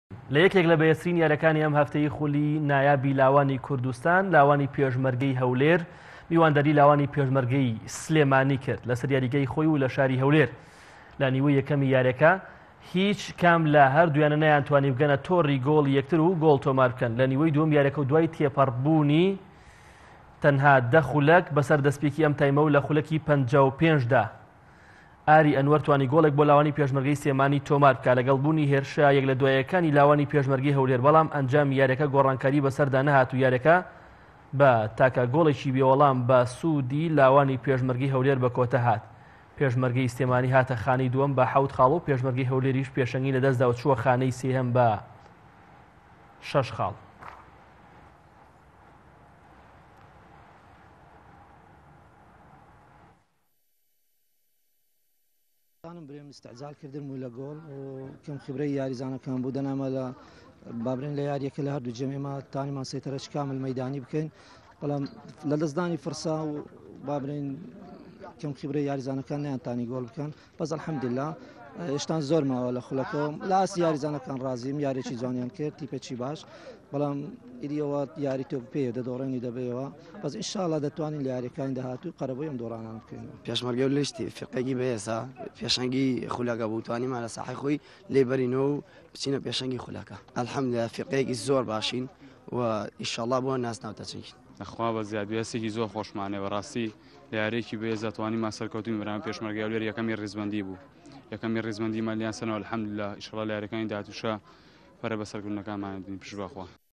خولی نایابی لاوانی كوردستان پێشمەرگەی سلێمانی سەركەوتنەكانی پێشمەرگەی هەولێری راگرت
خولی نایابی لاوانی كوردستان
پێشمەرگەی سلێمانی سەركەوتنەكانی پێشمەرگەی هەولێری راگرت